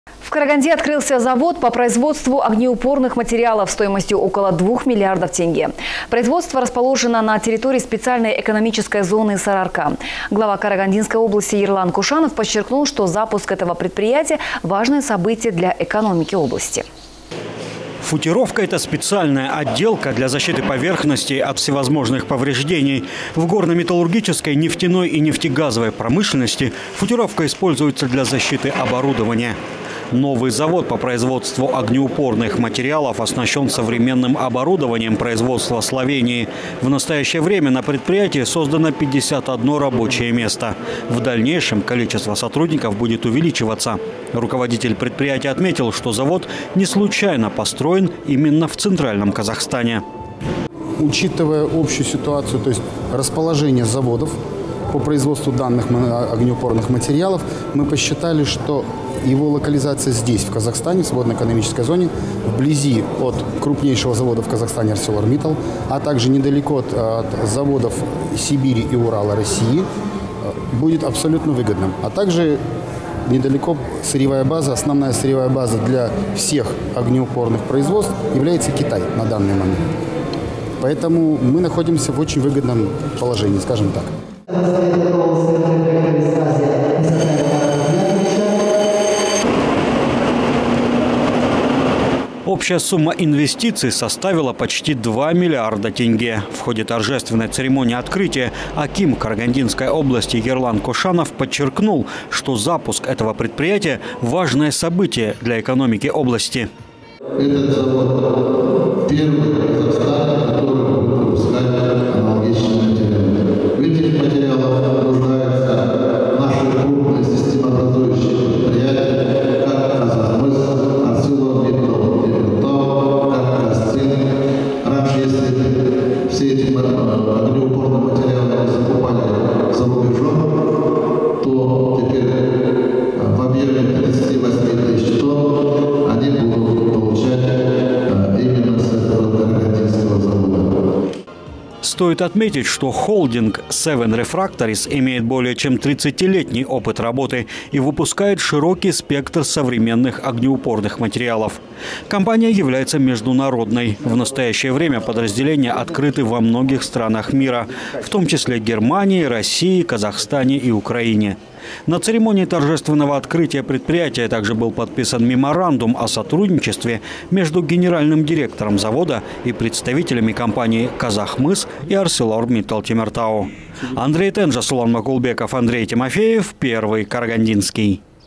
В КАРАГАНДЕ ОТКРЫЛСЯ ЗАВОД ПО ПРОИЗВОДСТВУ ОГНЕУПОРНЫХ МАТЕРИАЛОВ
В КАРАГАНДЕ ОТКРЫЛСЯ ЗАВОД ПО ПРОИЗВОДСТВУ ОГНЕУПОРНЫХ МАТЕРИАЛОВ СТОИМОСТЬЮ ОКОЛО ДВУХ МИЛЛИАРДОВ ТЕНГЕ. Производство расположено на территории специальной экономической зоны «Сарыарка». Глава Карагандинской области Ерлан Кошанов подчеркнул, что запуск этого предприятия – важное событие для экономики области.

Футеровка – это специальная отделка для защиты поверхностей от всевозможных повреждений. В горно-металлургической, нефтяной и нефтегазовой промышленности футеровка используется для защиты оборудования. Новый завод по производству огнеупорных материалов оснащен современным оборудованием производства Словении. В настоящее время на предприятии создано 51 рабочее место, в дальнейшем число сотрудников будет увеличиваться. Руководитель предприятия отметил, что завод не случайно был построен в Центральном Казахстане.

Общая сумма инвестиций составила почти два миллиарда тенге. В ходе торжественной церемонии открытия аким Карагандинской области Ерлан Кошанов подчеркнул, что запуск этого предприятия - важное событие для экономики области.

Стоит отметить, что холдинг «Сэвен Рефракторис» имеет более чем 30-летний опыт работы и выпускает широкий спектр современных огнеупорных материалов. Компания является международной - в настоящее время подразделения открыты во многих странах мира. В том числе в Германии, Украине, России и Казахстане. На церемонии торжественного открытия предприятия также был подписан меморандум о сотрудничестве между генеральным директором завода и представителями компаний "Казахмыс" и "АрселорМиттал Темиртау".